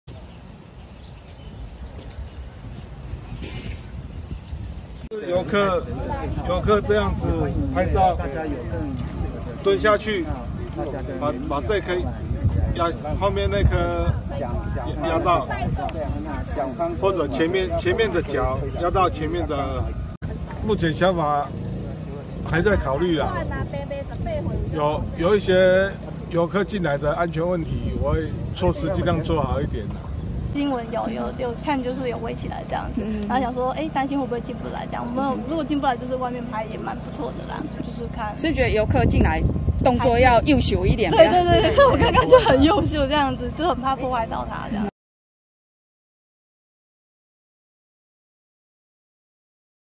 「台版波波草」被壓出大洞 田尾祕境不再任你行
彰化縣田尾鄉一處農田種有4000株細葉七里香，因圓滾滾的造型充滿療癒，被封為「台版波波草」祕境，吸引不少網友朝聖打卡，最近傳出有民眾為了搶拍，不慎踐踏，造成不少波波草被壓出洞來，1個月來已遭毀損50株，園主夫妻相當心疼，只好在園區周邊架起鐵網圍籬，讓民眾擔心，祕境是否要封園？園主張至辰表示，沒有要封園，但不堪毀損，未來進出遊客園區將朝收費或限量管制。